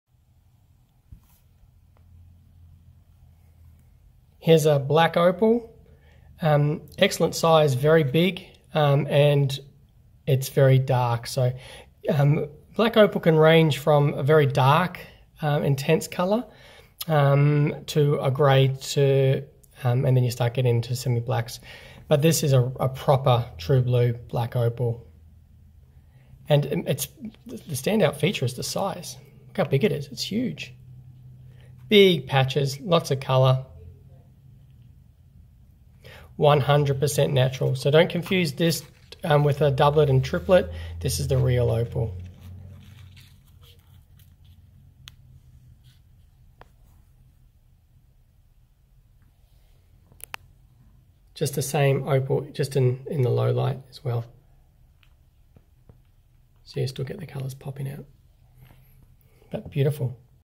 Blue Opal Pendant
Black Opal Stone - Blue Pendant - Huge it has real opal stone, blue is the popular colour, beautiful piece of jewellery. For Natural Opal Jewellery from the website in the featured video, please go For information about opal or visit the Opal Museum in Brisbane Australia go